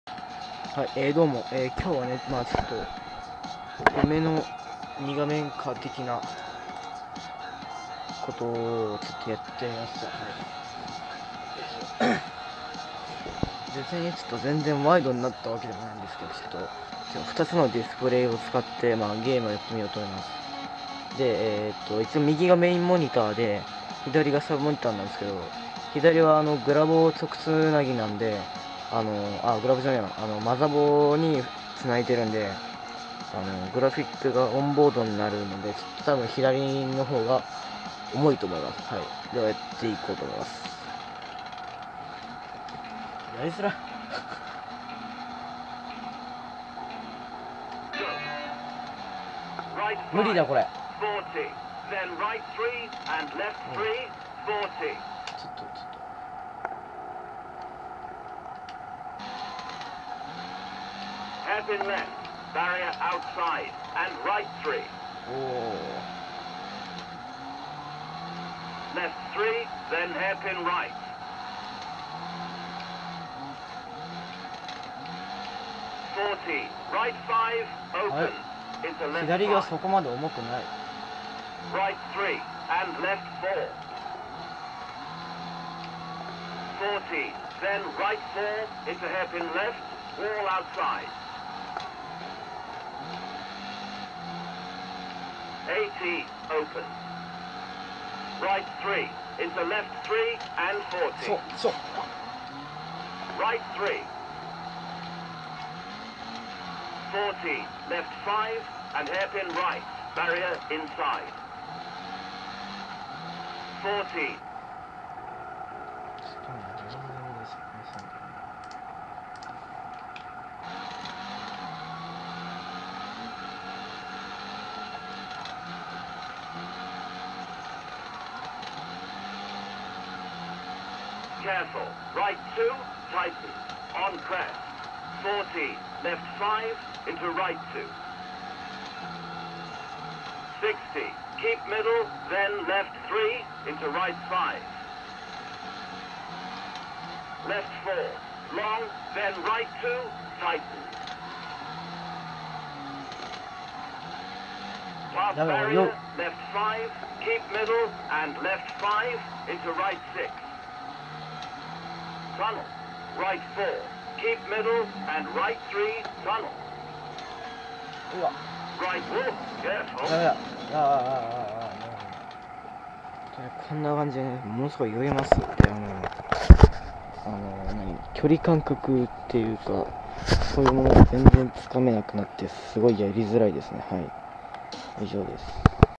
デュアルディスプレイでゲームやってみたｗｗｗｗｗｗｗｗｗｗｗ
死ぬほど酔う　死ぬほどやりずらい